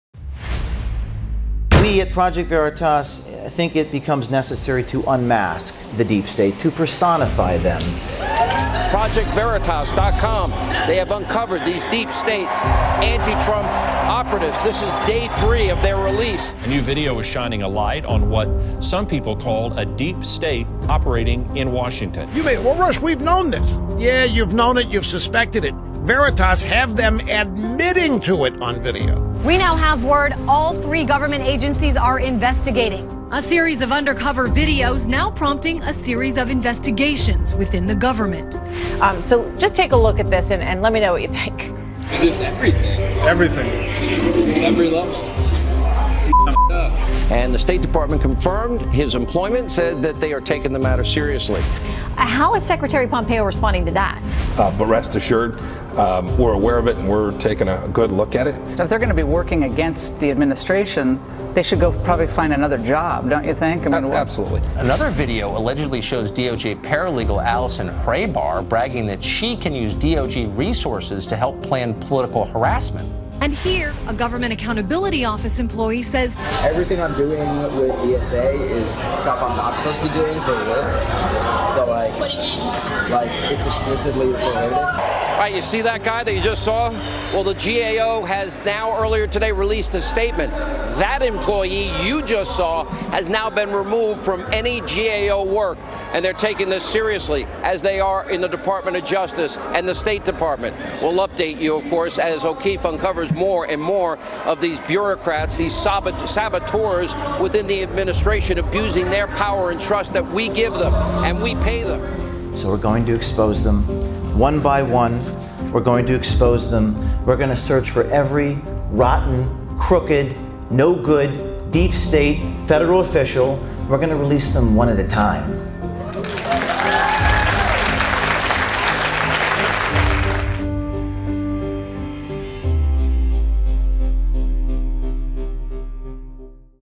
Rocking the Swamp: Agency Wide Resistance (ON TAPE) May Violate Anti-Conspiracy Laws Against US
A highlight reel from the first week of Project Veritas' Deep State Unmasked series.